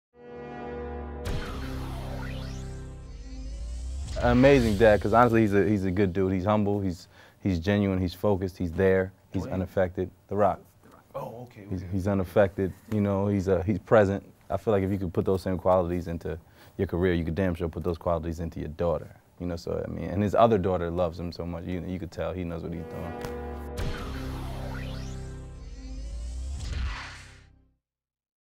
'Ballers' Star, Antoine Harris Reacts To The Rock Welcoming His Second Child
HipHollywood caught up with 'Ballers' star, Antoine Harris, to talk about his co-star Dwayne 'The Rock' Johnson welcoming a baby girl into the world.